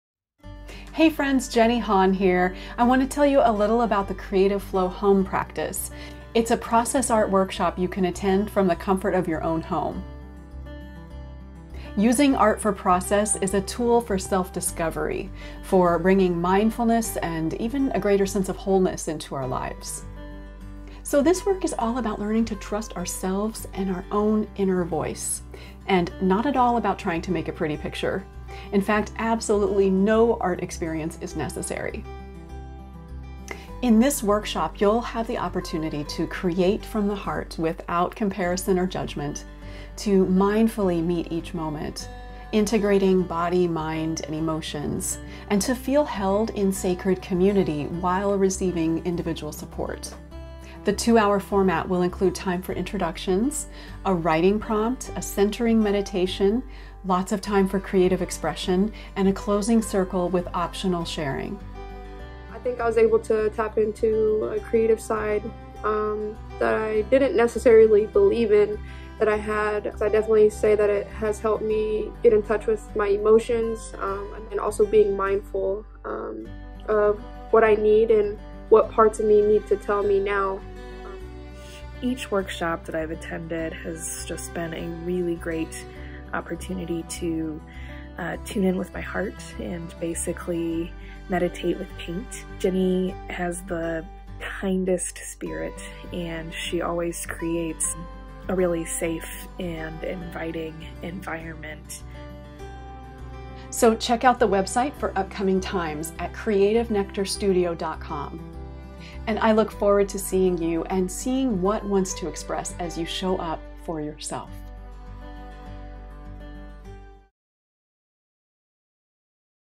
Creative Flow Home Practice, with Jenny Hahn
You are a creative being worthy of expression. Find out more about this virtual workshop offered from the comfort of your own home. Artist and facilitator Jenny Hahn will guide you in tapping your inner creativity and expressing from your heart.